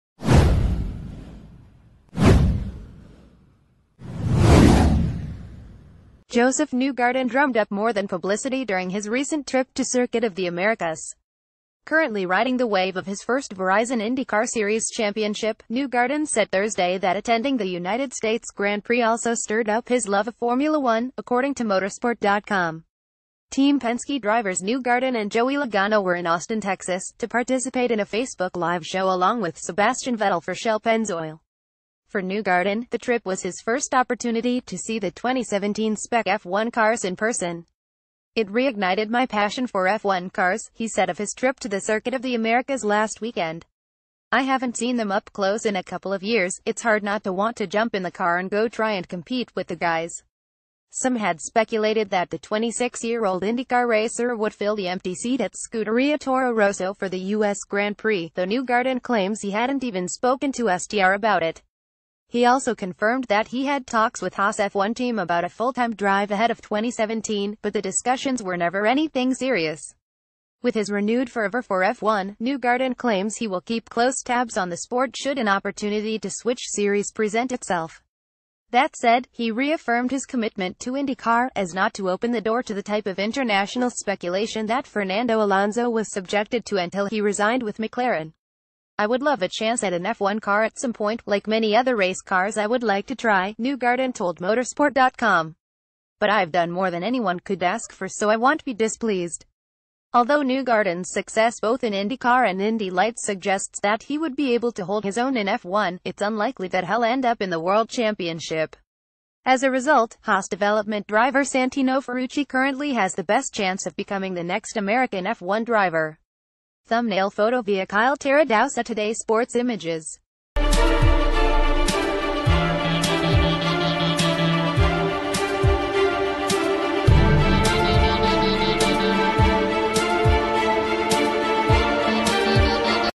F1 | After us grand prix trip, josef newgarden ‘would love’ to test f1 car
After us grand prix trip, josef newgarden ‘would love’ to test f1 car

Josef Newgarden drummed up more than publicity during his recent trip to Circuit of the Americas. Cu...